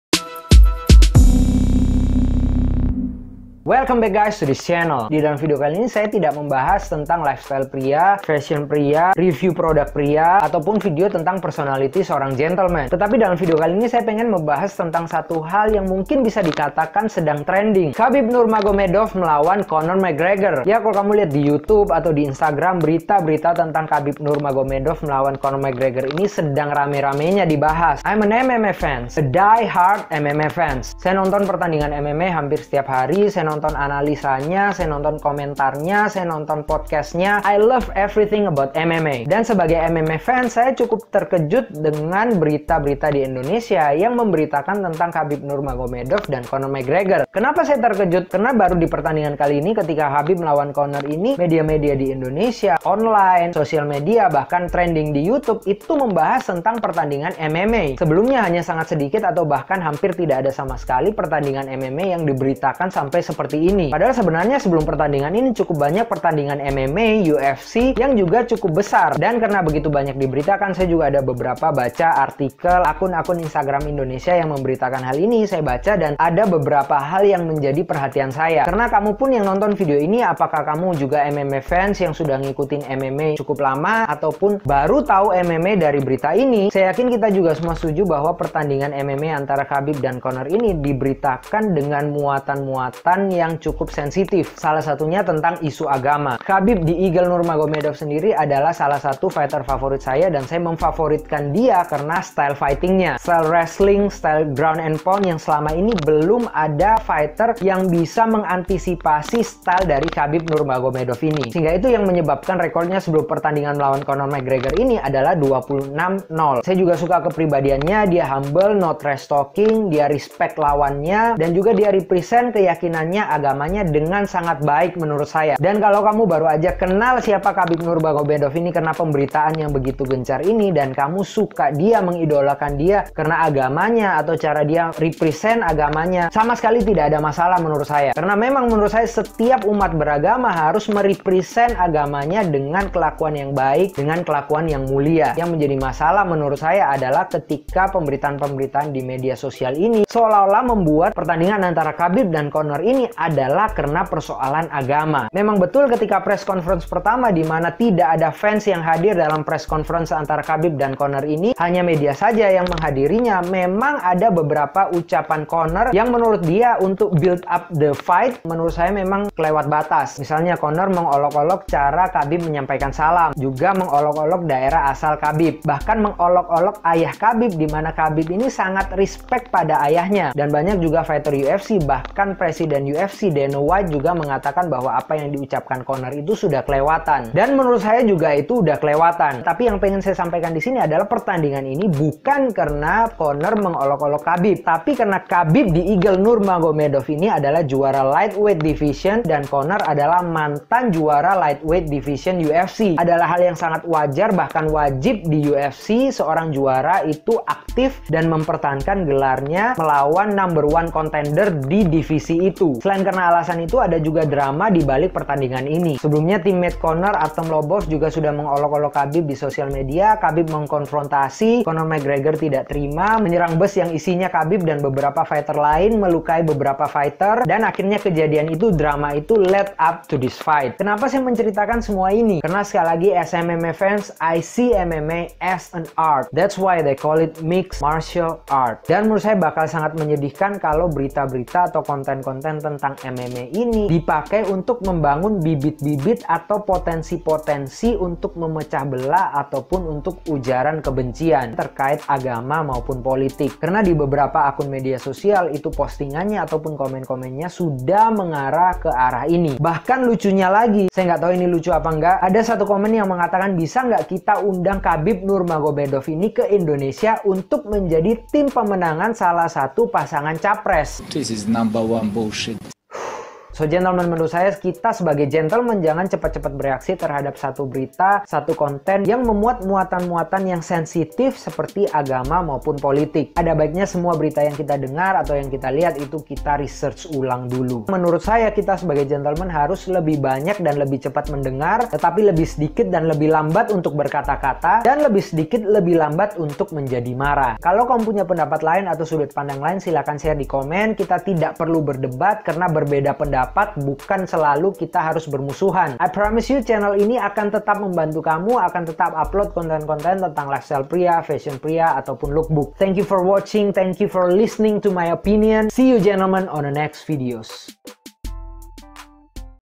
SIAPA SIH DIA ?? | Pendapat Saya Mengenai Khabib Nurmagomedov vs Conor McGregor
Mohon ditonton dulu sampai habis sebelum memberikan jawaban atau komentar.

Semoga video singkat ini bermanfaat !

and KEEP MOVING FORWARD !!